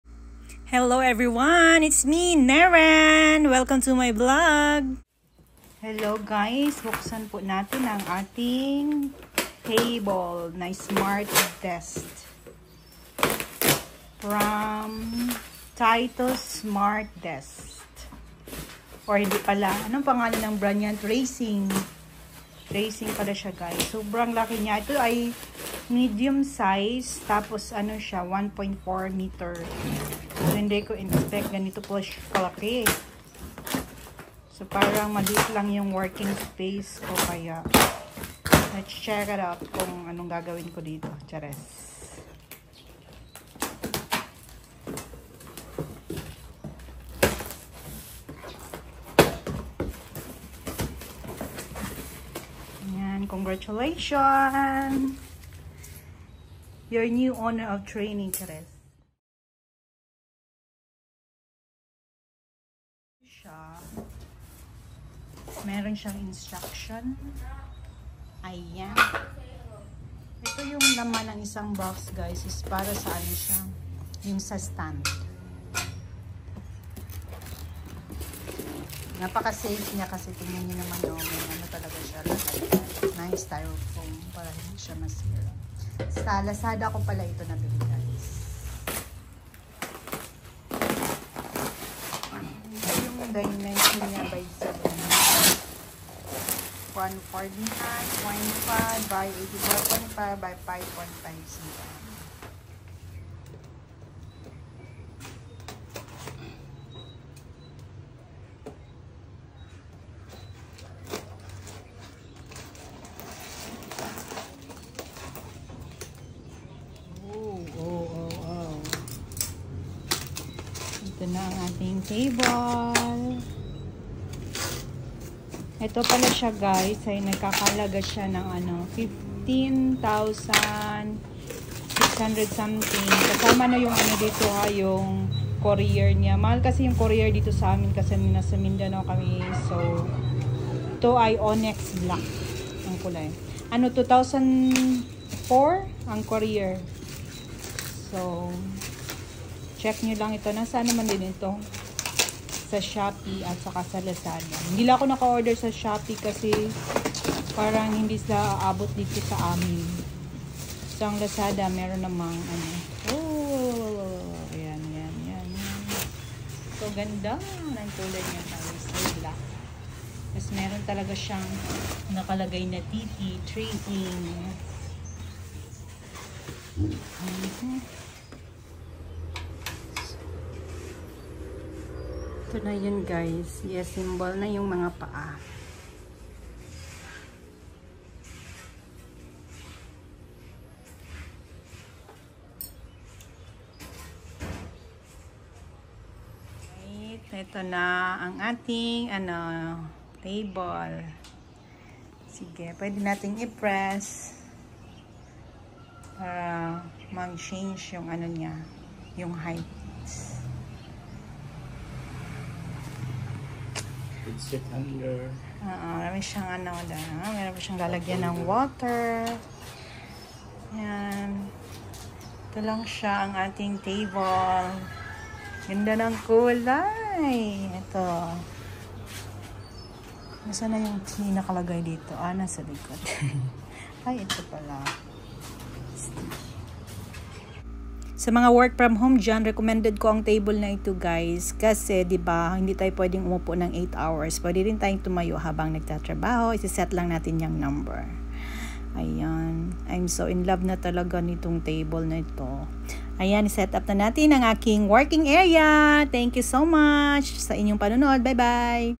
Smart desk for work from home
Maganda po Ang table at durable siya. Recommended ko to sa mga work from home ❤️